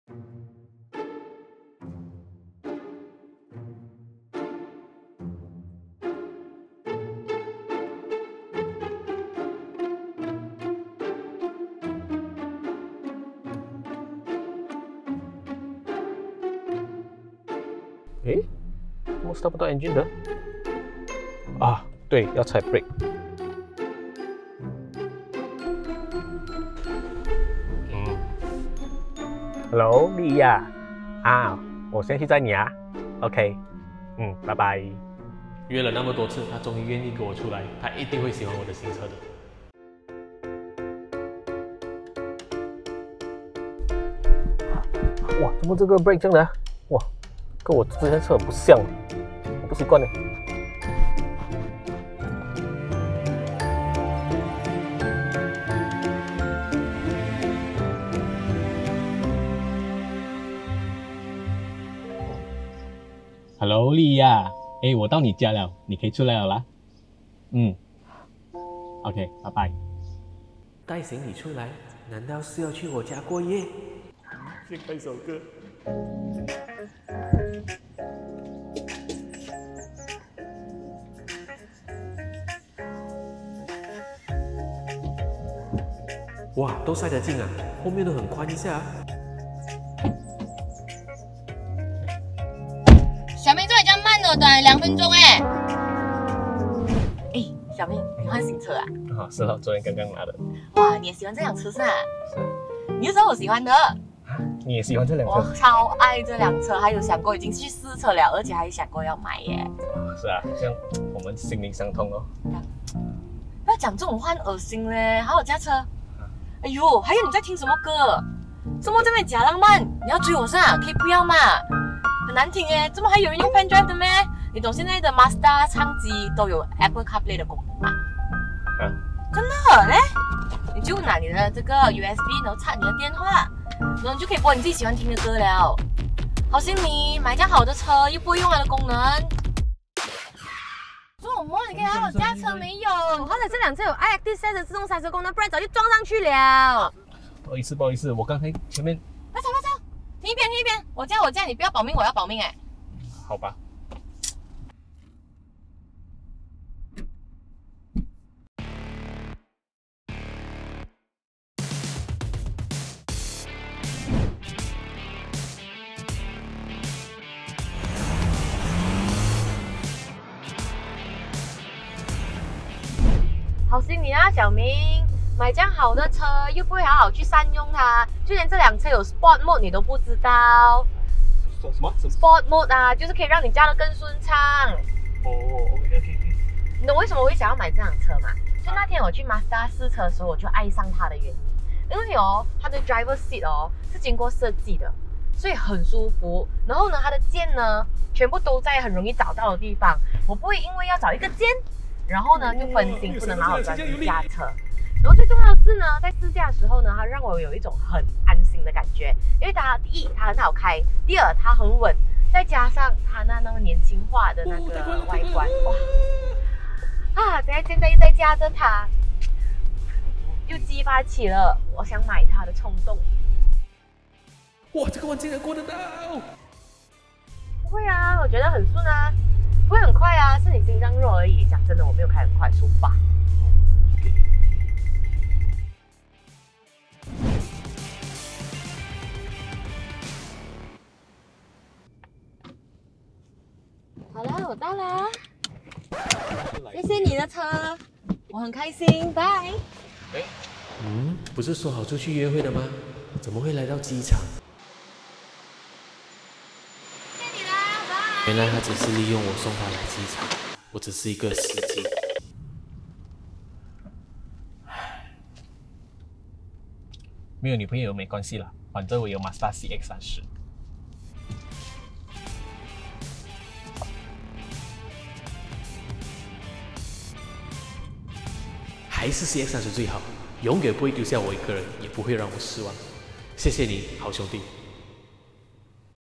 小苠用 2020 Mazda CX-30 追女神，结果。。。｜automachi.com 马来西亚试车频道（中文字幕）
在约了大概382.5次以后，小苠心仪已久的女神俪怡终于答应和他出门了！为了慎重其事，小苠这回使出了杀手锏，选择了颜值和女神一样高的全新 2020 Mazda CX-30 出门接送女神约会，希望能够让自己再加分。结果好像还真的凑效耶，因为原来俪怡也很喜欢这一辆 Mazda 最新的精品 Crossover！

就在小苠觉得胜券在握，感觉已经到达人生巅峰的时候，才发现原来俪怡竟然比自己还了解这款基于 Mazda 最新 SkyActiv Vehicle Architecture 车体架构打造而成的跨界休旅车！女神不但熟悉地操作 CX-30 全车系所标配的 Apple CarPlay 功能，还把车子的极限发挥得淋漓尽致，而小苠却在这个关键时刻让 CX-30 所搭载的 i-ACTIVSENSE 先进主动式安全系统里的自动紧急刹车功能派上用场，唉。。。我们都没有眼睛看下去了。。。

不说了，大家还是看影片欣赏女神吧！大家觉得俪怡美不美呢？❤️